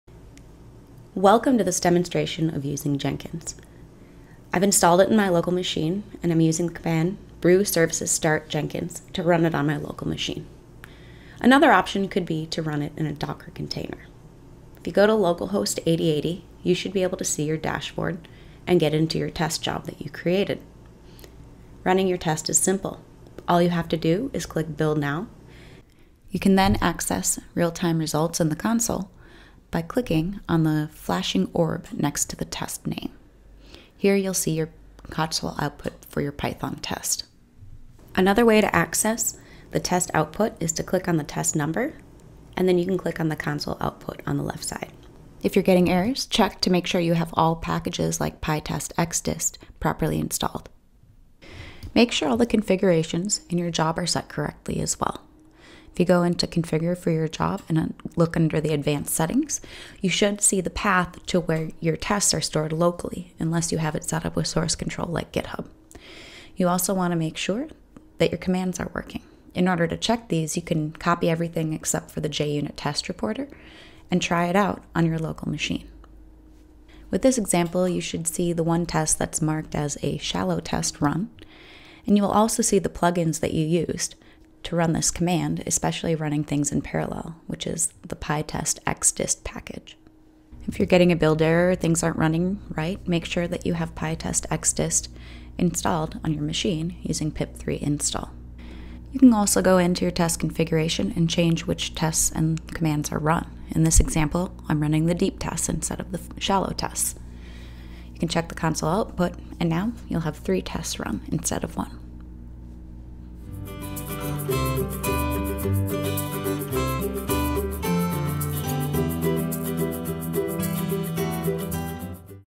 Using Jenkins with a Python Test
This video will show you how to run Jenkins with Homebrew and Python. Get a walk through of the Jenkins interface  how to run and check the configuration of your test job with the JUnit reporter for Jenkins.

This video is created as a part of Sauce Labs open-source Selenium Python